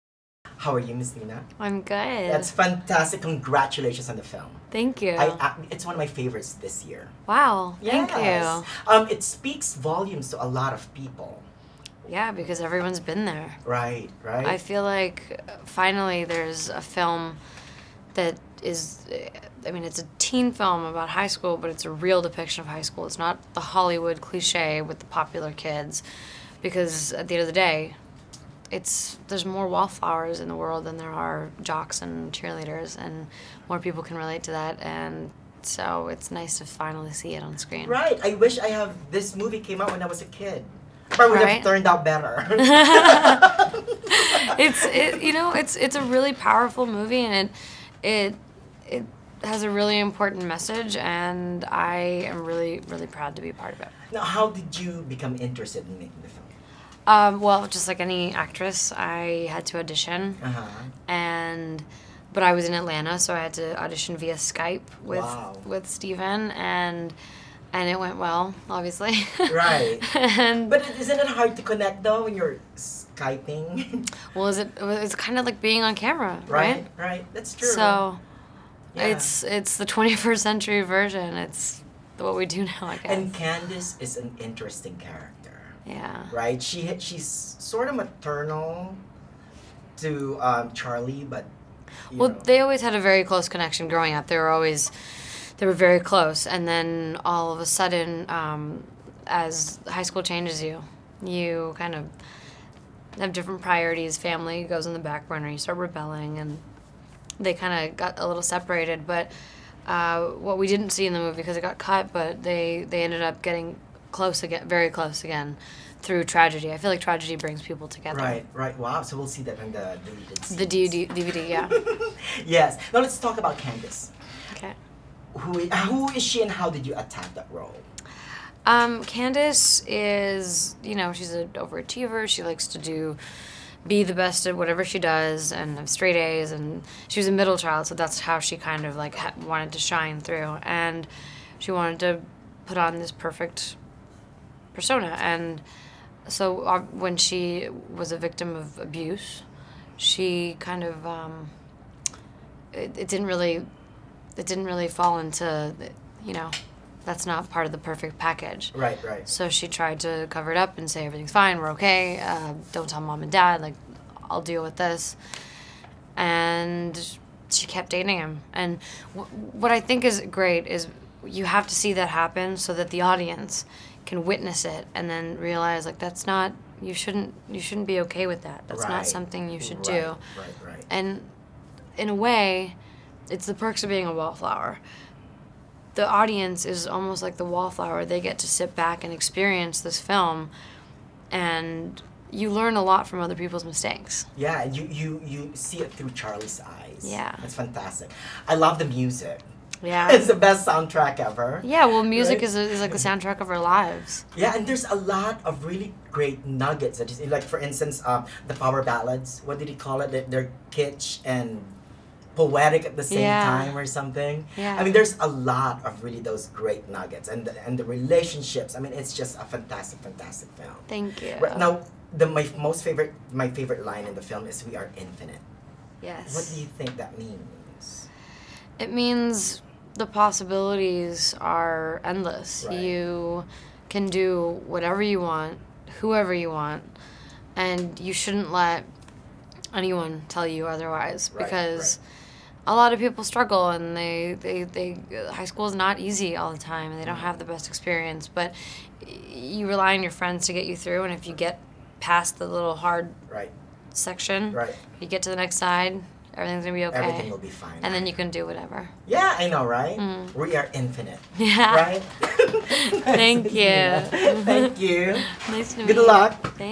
Nina Dobrev Interview for "The Perks of Being a Wallflower"
Nina Dobrev, "The Vampire Diaries" star, is part of the fantastic ensemble of the new film "The Perks of Being a Wallflower."  She plays Candace, the sister of our wallflower, Charlie (Logan Lerman).  Emma Watson and Ezra Miller co-star from author/writer/director Stephen Chbosky.  I met the actress in Toronto for the 2012 TIFF for this interview.  We talked about:

Why does the book resonate to many people?
How did she get involved into making "Perks of Being a Wallflower?"
Her character and her bond with Charlie
What you can expect in the DVD's deleted scenes
Music as the soundtrack of our lives
The meaning of "we are infinite"

Have fun!